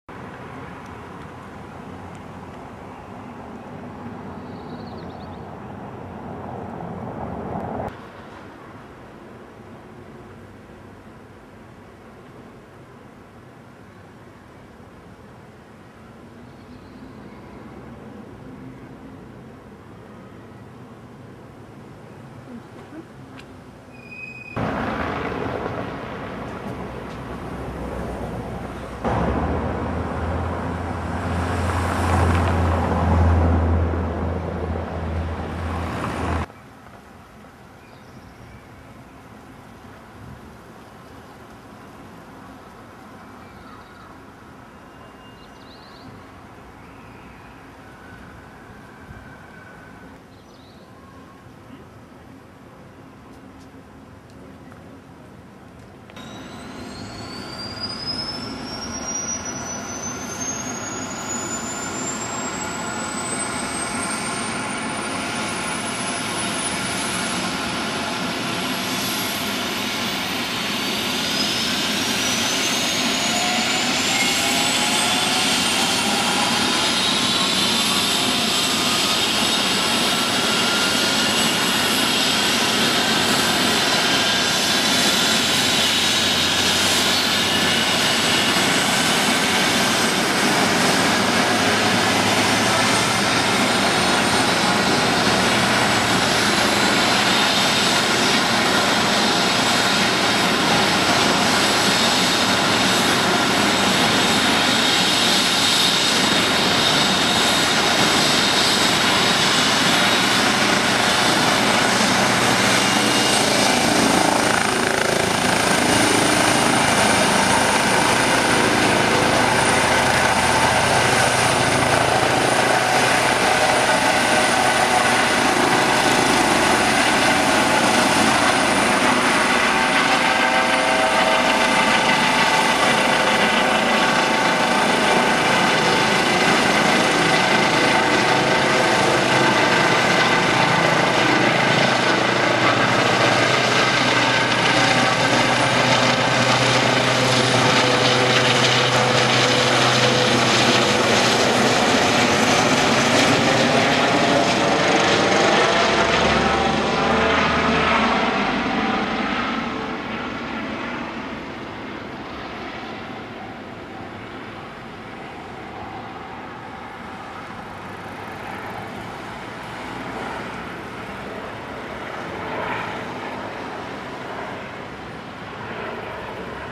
OeAMTC-Helicopter in Kagran, 1220 Wien
Ein kurzer Einsatz eines ÖAMTC-Helicopters (OE-XEN) bei der Albert-Schultz-Eishalle in Kagran (1220 Wien). Die Crew flog scheinbar unverrichterter Dinge und ohne Patient wieder ab.